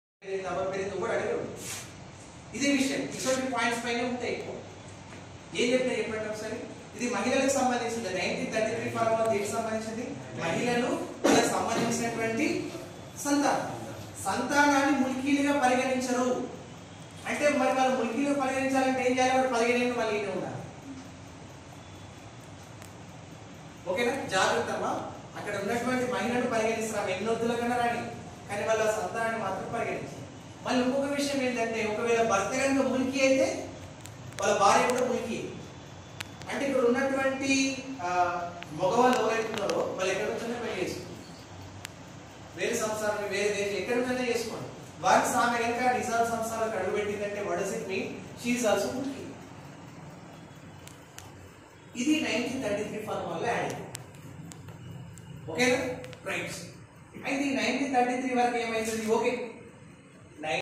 Telangana movement class in A Plus Academy in Siddipet by Naveen Sir.
SI& CONSTABLE